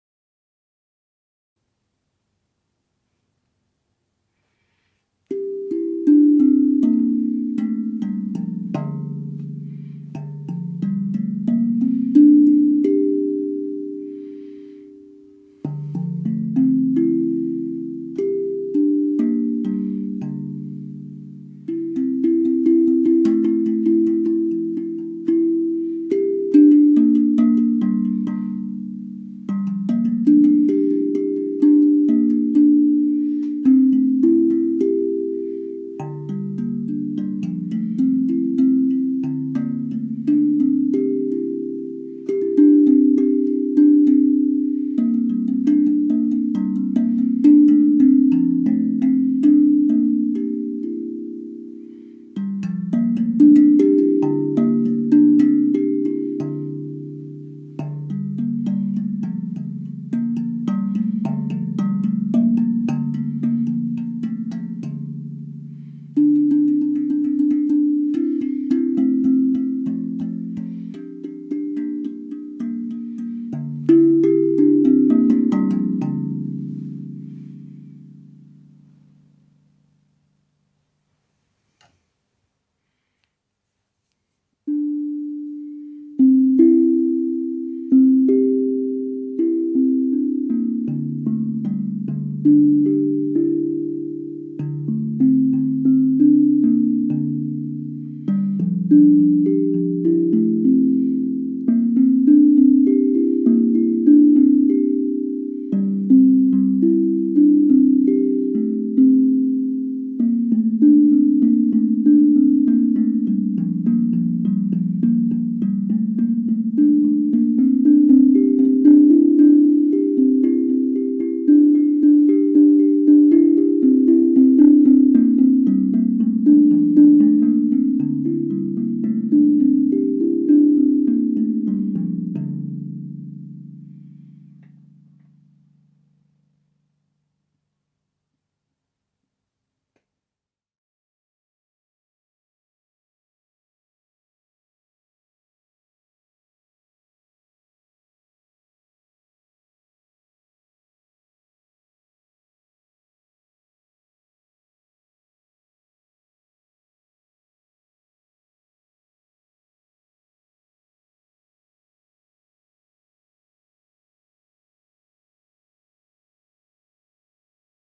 Kool Drum NOVA, Quénécan tuning
Kool Drum Nova, 9 notes. Please listen with headphones or speakers.
Quénécan tuning    (D3)   F3   G3   A3   Bb3   C4   D4   E4   G4
for more information and contact